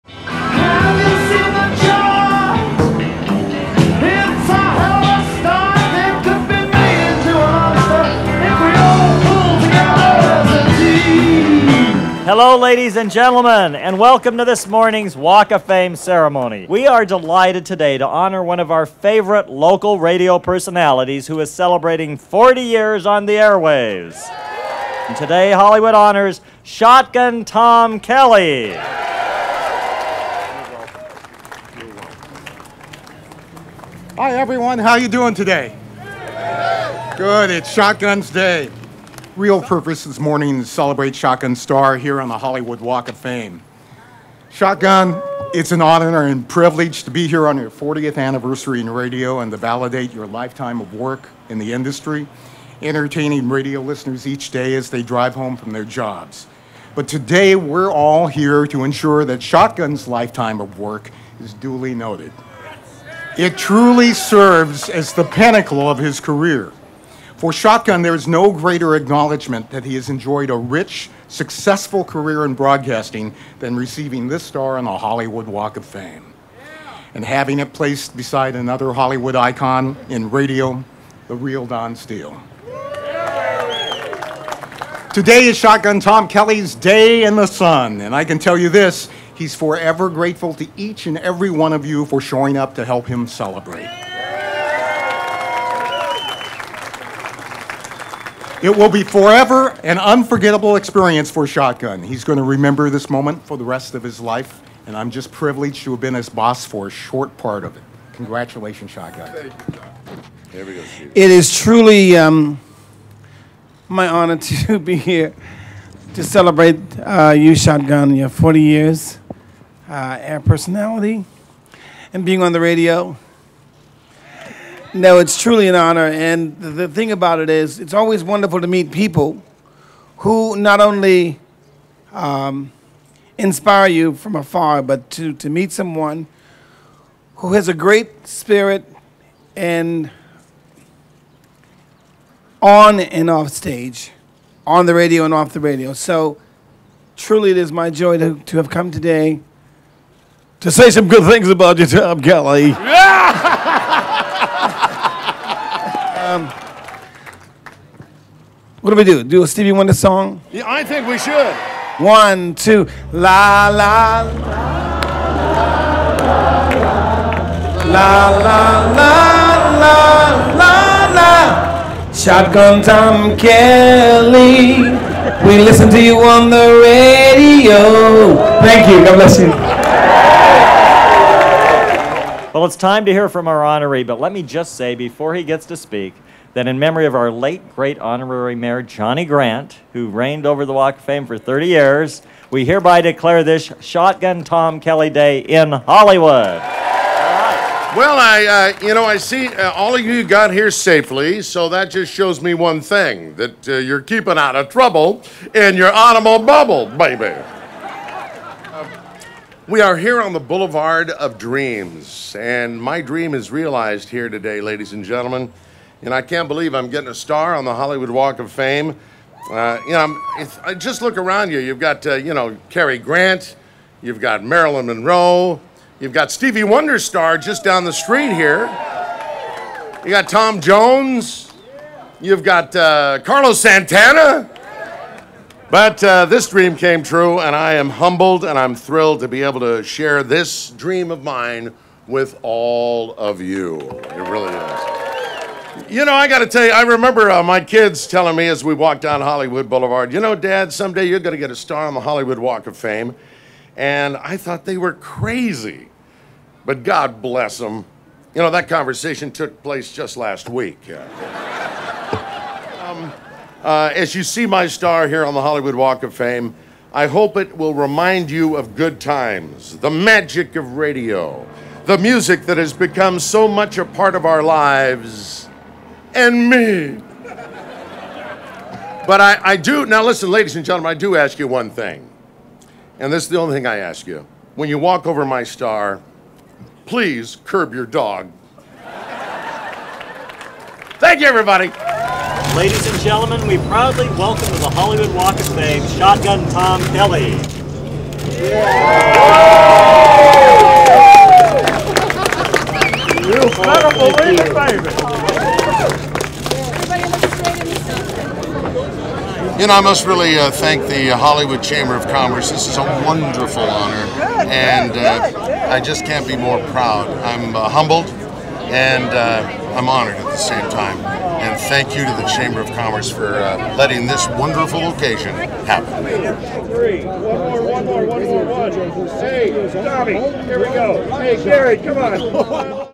"SHOTGUN TOM" KELLY HONORED WITH HOLLYWOOD WALK OF FAME STAR
Radio personality "Shotgun Tom" Kelly was honored with the 2,496th Star on the World Famous Hollywood Walk of Fame, April 30, 2013, in Los Angeles.